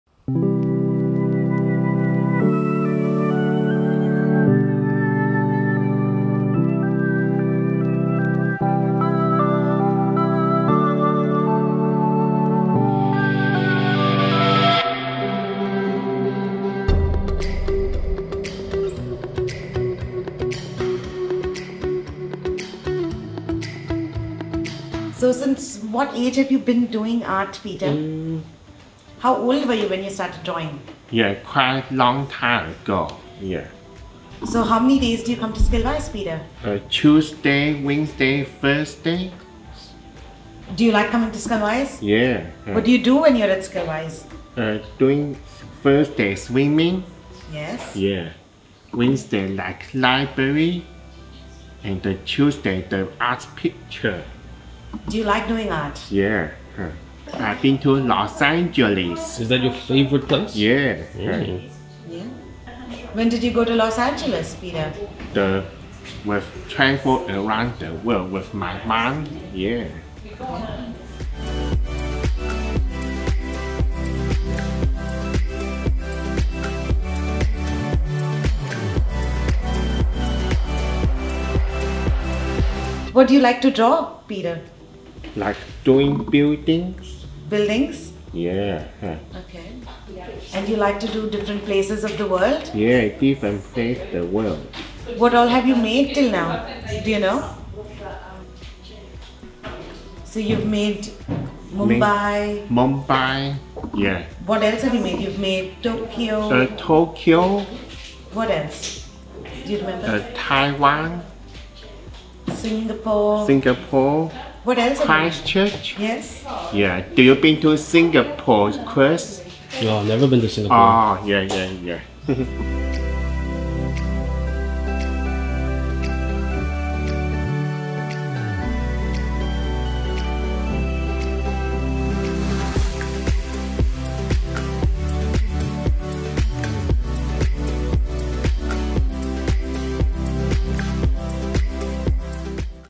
hello, Chou
Peter Chou has a solo exhibition 3-8 Feb 2020 at the XCHC Exchange Christchurch. See if create and hear him talk in this video